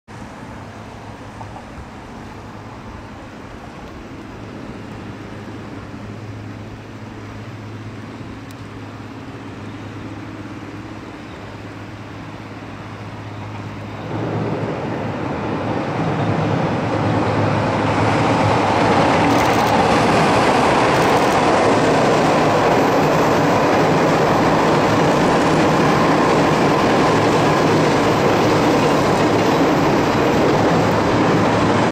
JR貨物 2014/10/26 EF66-131[吹]号機 次位新灰コキ107(1083~1088)6B連結 貨物列車5092レ
撮影日 2014年10月26日(日) AM7:59
撮影地 東海道本線・新居町~弁天島